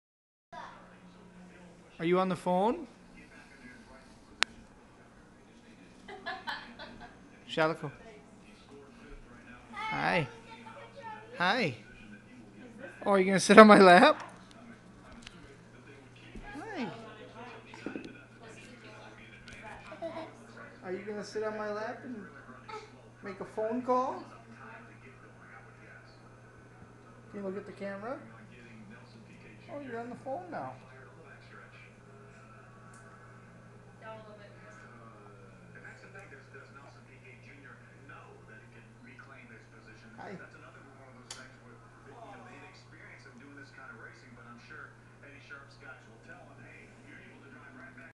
Shalikco.mov
My Niece Shalikco and I hanging out.